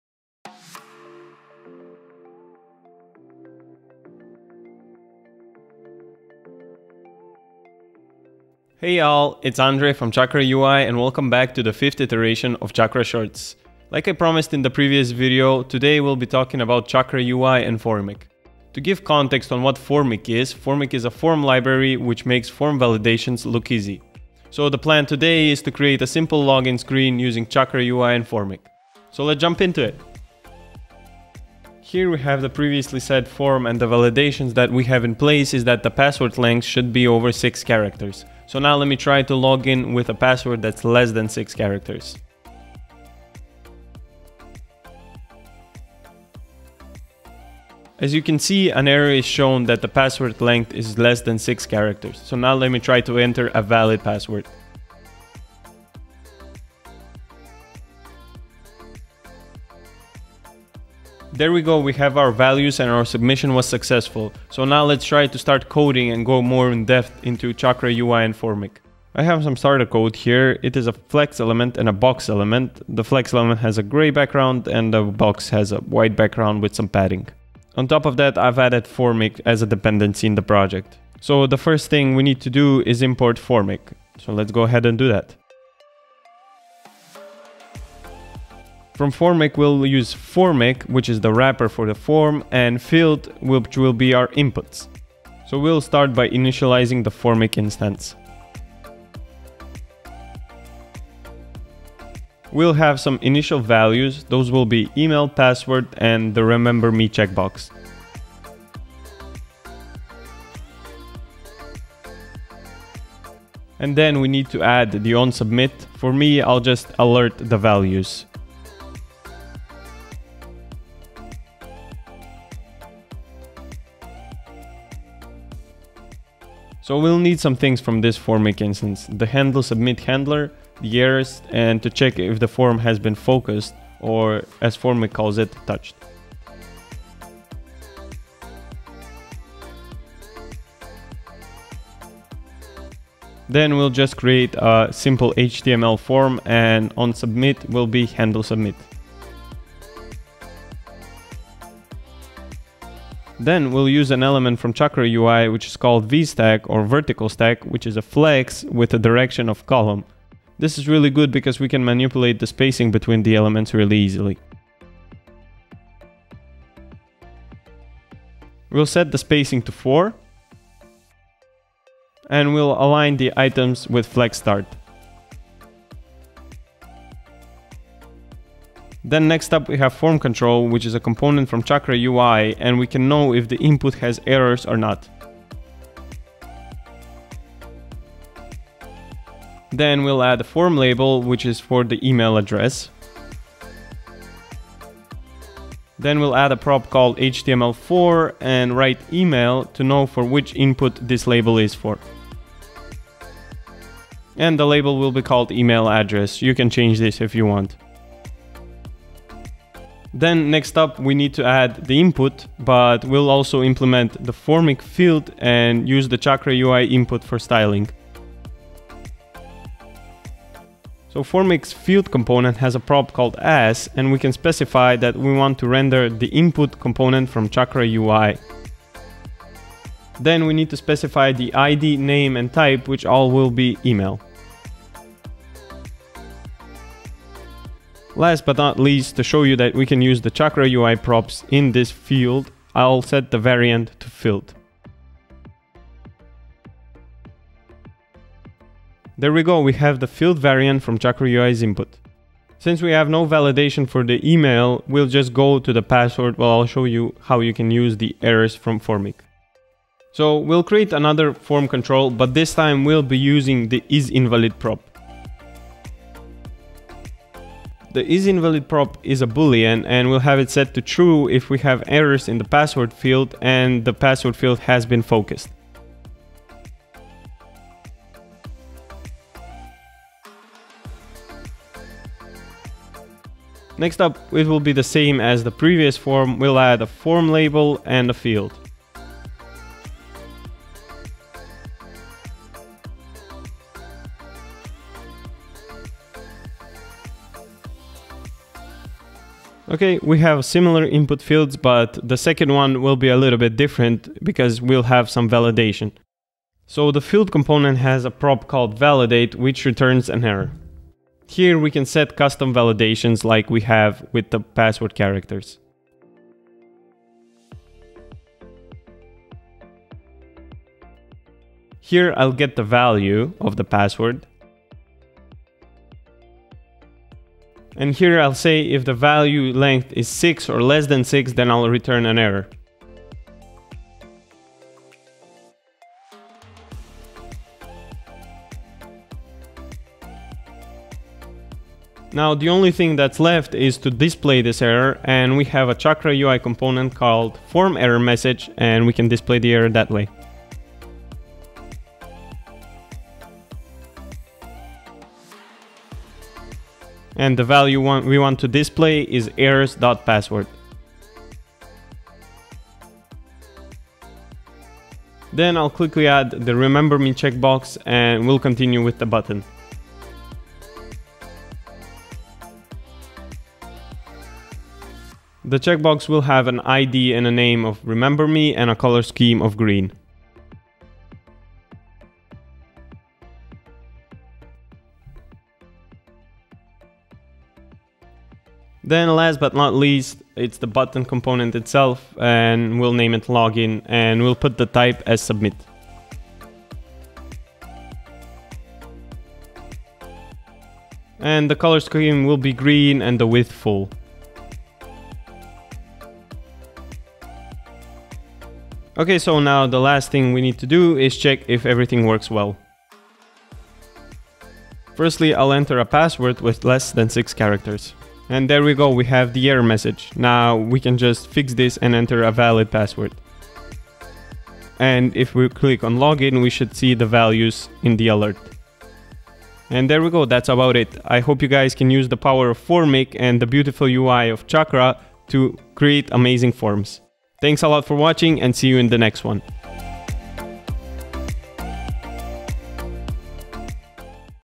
Chakra Shorts #5 - Chakra UI + Formik
In this episode of Chakra Shorts, we will talk more about how you can use the power of Formik with the beautiful UI of Chakra.